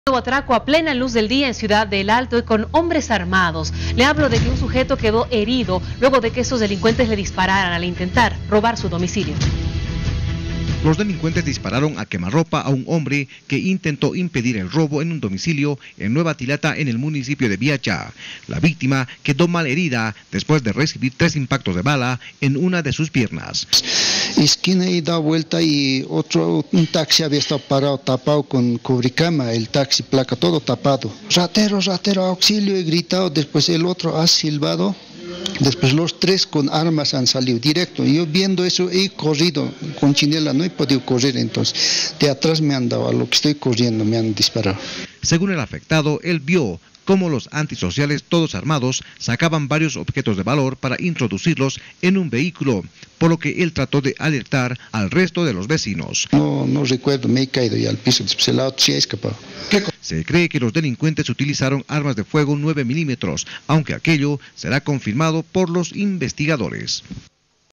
INTENTÓ EVITAR UN ATRACO Y LO HIRIERON
¡Lo hirieron por evitar un atraco! Un hombre resultó herido cuando delincuentes le dispararon. La víctima presenció cómo los ladrones se llevaron varios objetos de valor de una vivienda. (Video: PAT)